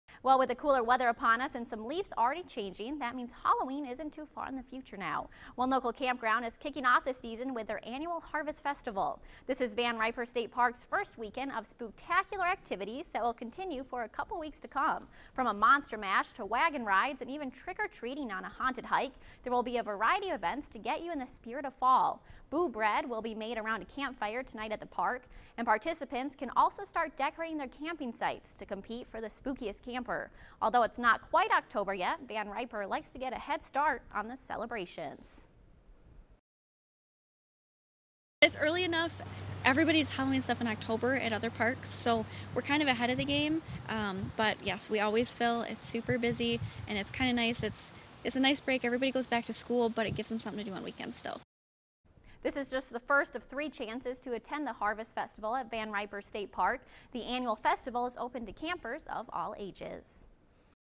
Local park holding early Halloween celebrations
This is Van Riper State Park's first weekend of spook-tacular activities that will continue for a couple weeks to come.